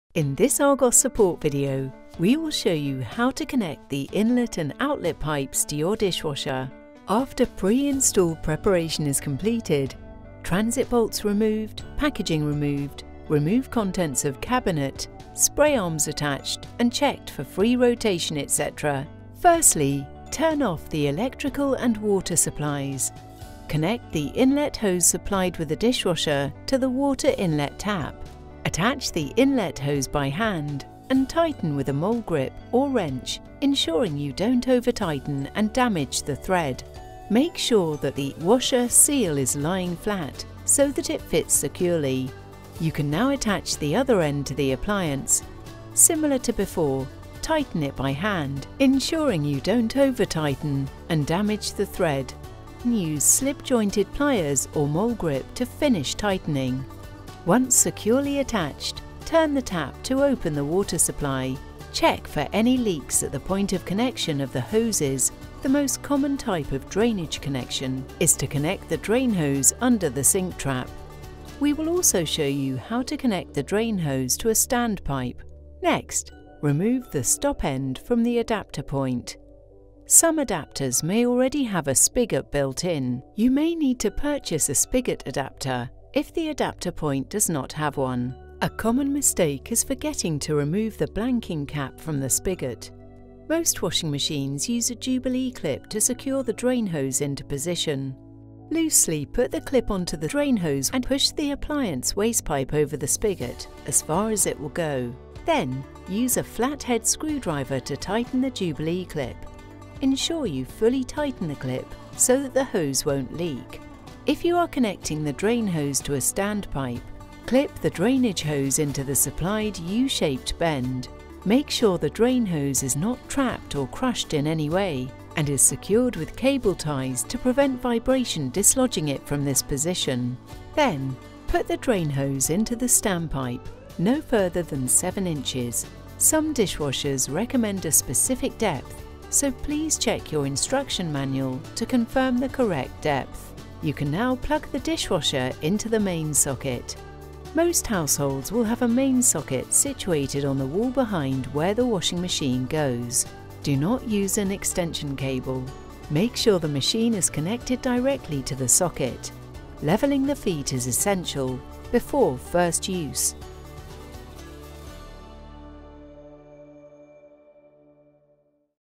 How to Connect a Dishwasher
In this Argos Support video, we will show you how to connect the inlet and outlet pipes on your dishwasher.